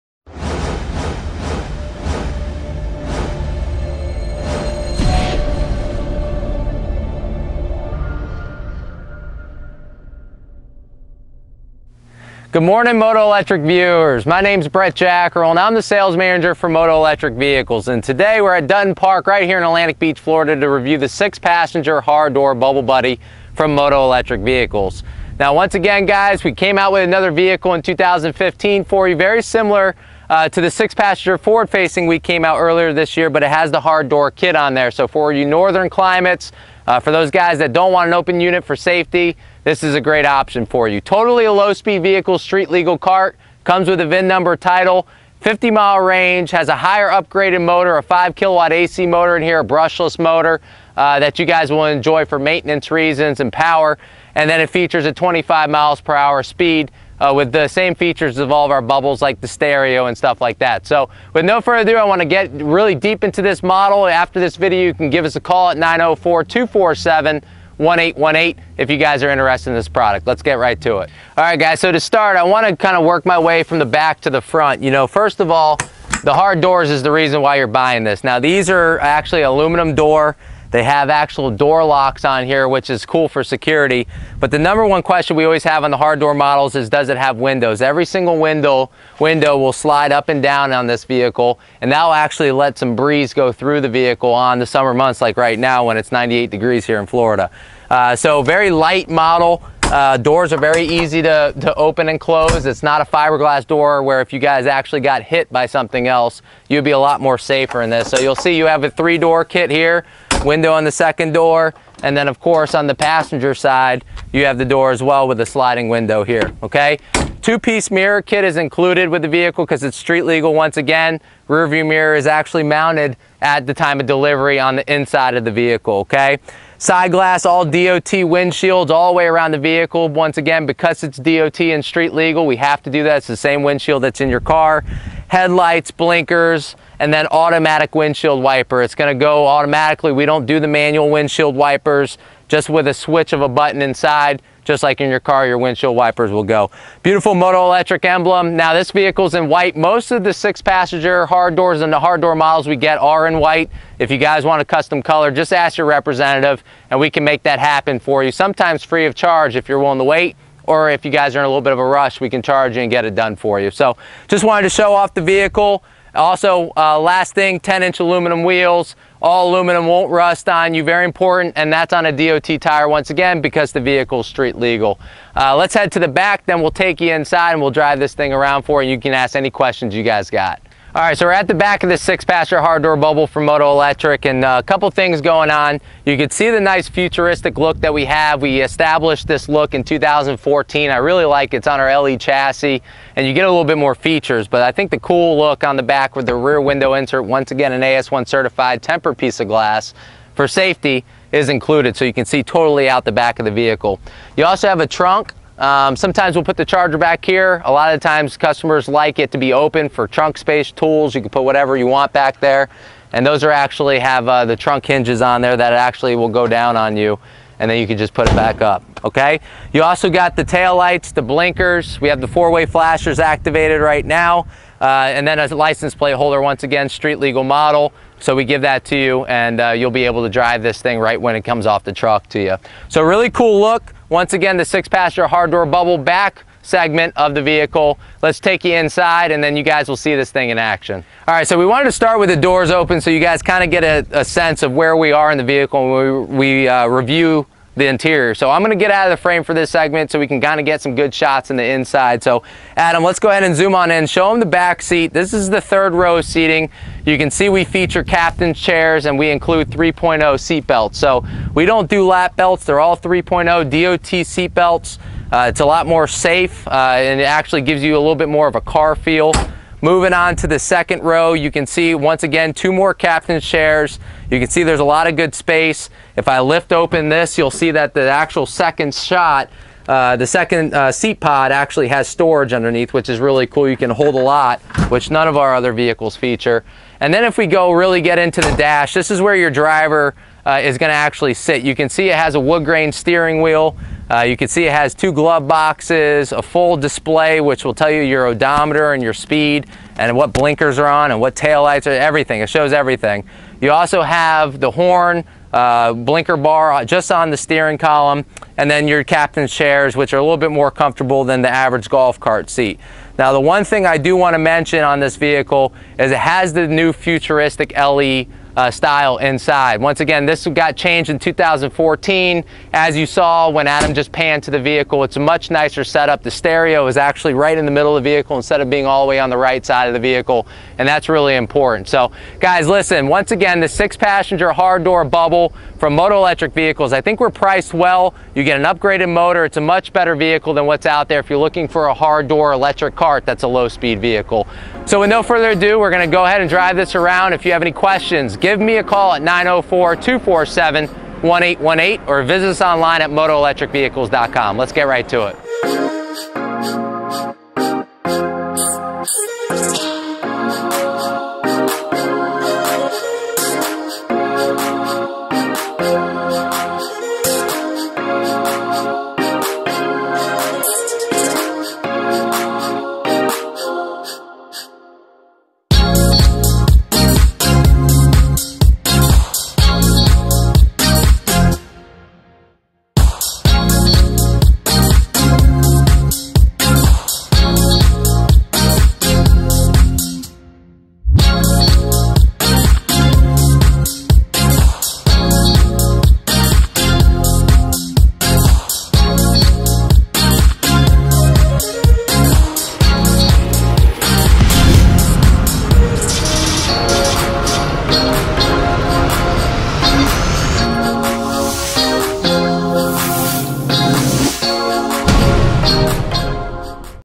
6 Passenger Hard Door Bubble Electric Cart- Low Speed Vehicles
Brett Jackrel from Moto Electric Vehicles reviews the brand new 2015 Low Speed vehicle 6 passenger Hard Door Bubble Buddy. This vehicle is street legal and gives the consumer a little better vehicle than the average street legal golf cart. Featuring Hard Doors, Stereo, 3.0 Seat Belts, and an extended body; this low speed vehicle is perfect for cold weather climates and rainy areas.